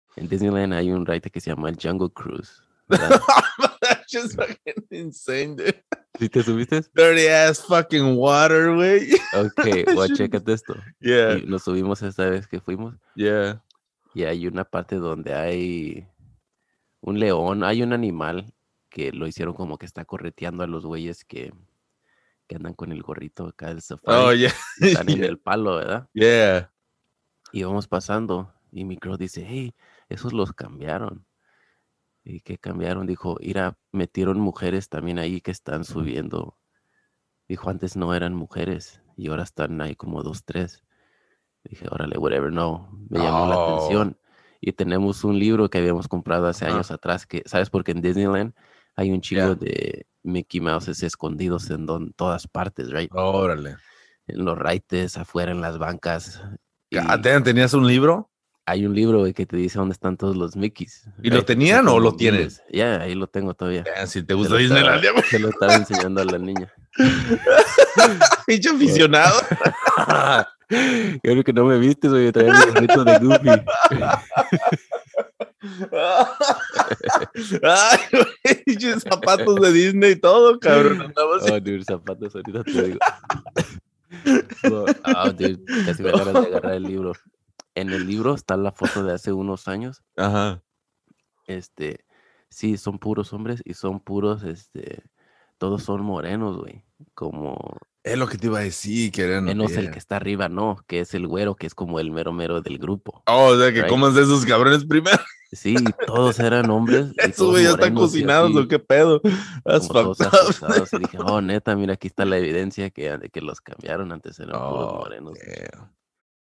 Disneyland Remodela Atracción Controversial 🐭
Durante una reciente visita al parque de Disney, la Sra. Bigoton notó cambios a la popular atracción “The Jungle Cruise”